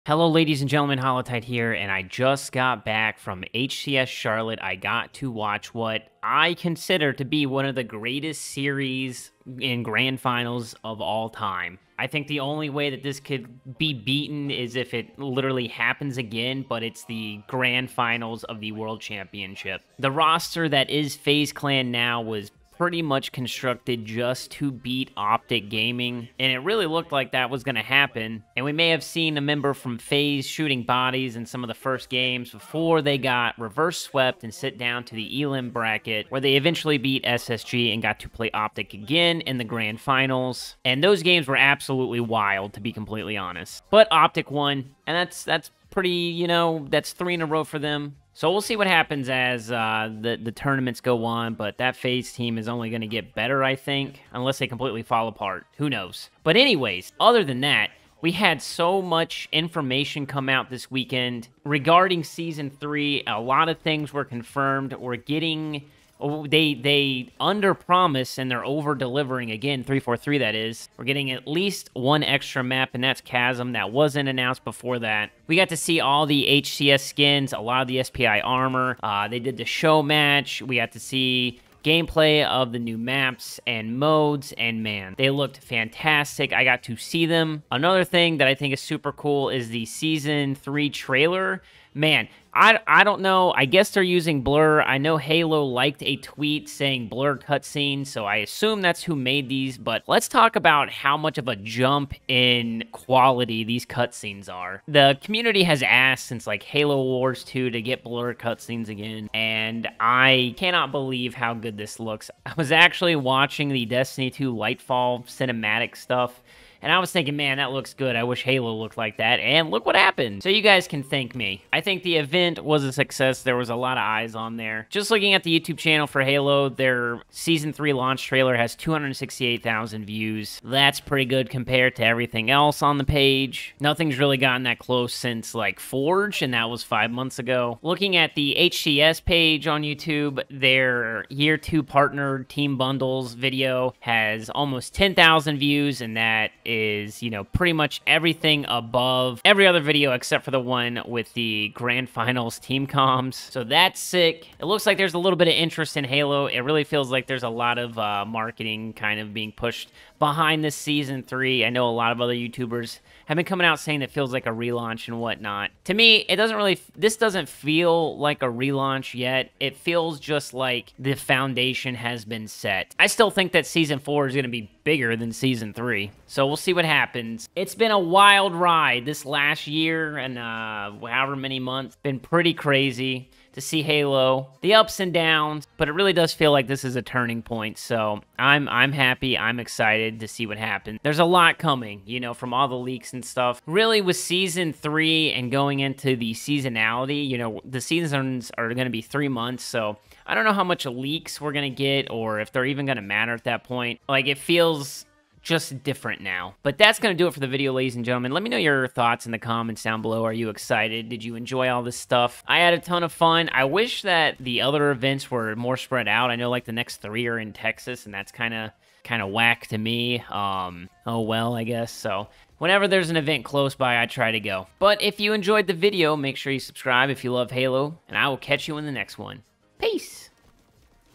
This Weekend Was A Turning Point For Halo Infinite
HCS Charlotte is in the books, with Optic Gaming winning the whole thing. It was a crazy Grand Finals, but more than that, we got a TON of Season 3 info and showcases for Halo Infinite. This felt like a huge turning point for the game.